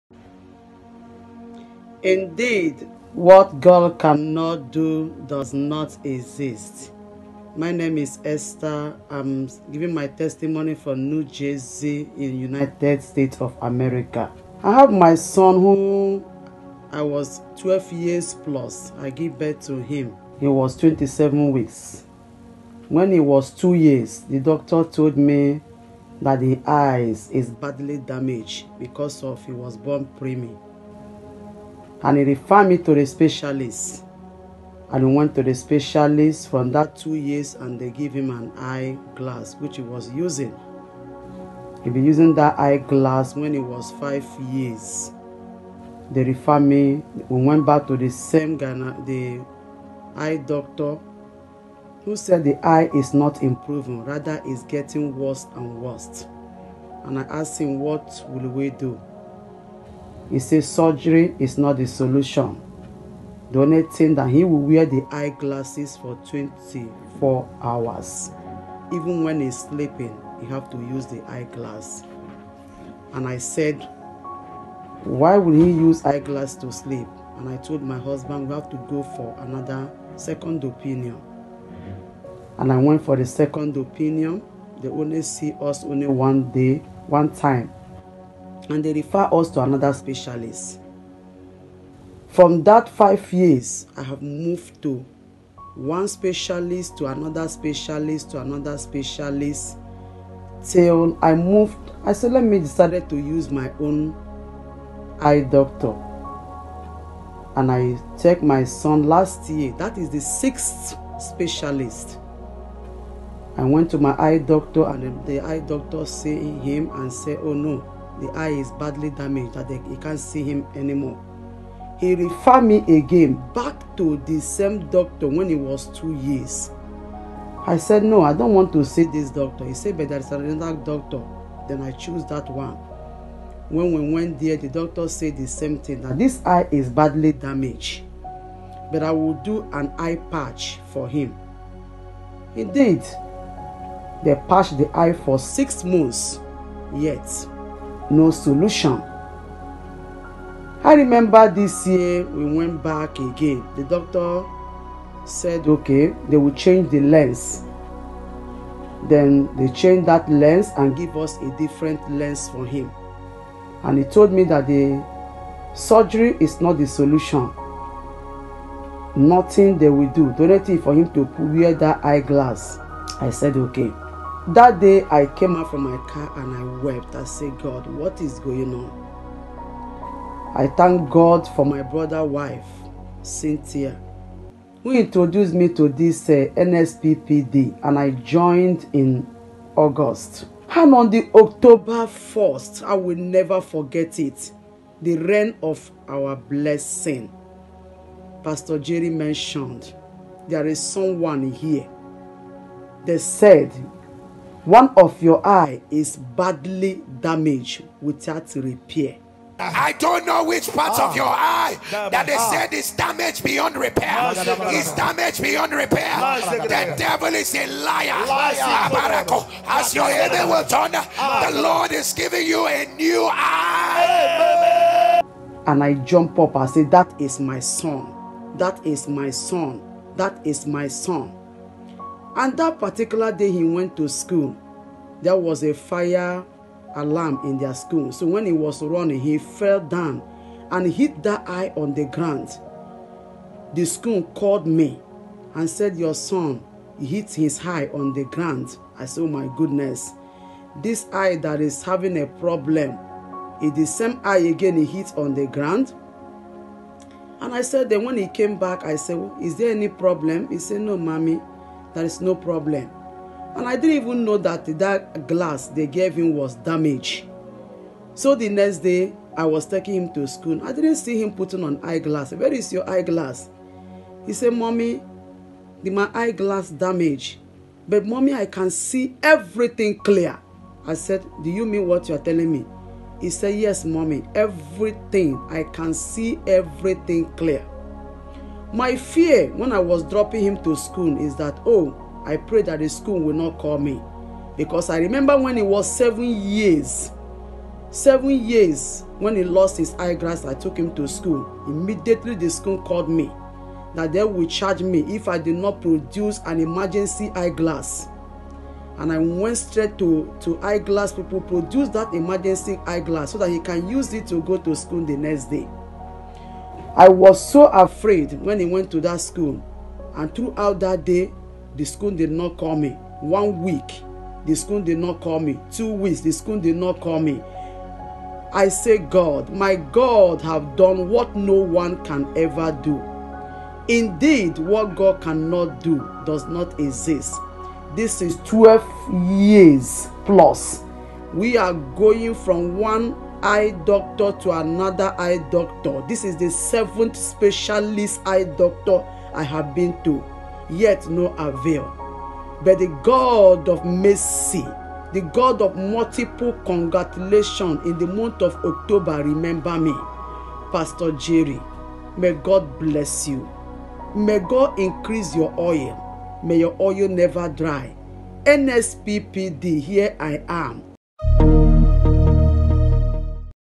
DOCTORS SAID HIS EYE WAS DAMAGED BEYOND REPAIR! JESUS FIXED IT AFTER A FALL.
Strange Acts of God
Testimonies from the New Season Prophetic Prayers and Declarations [NSPPD] that would boost your faith and make you believe God for the impossible.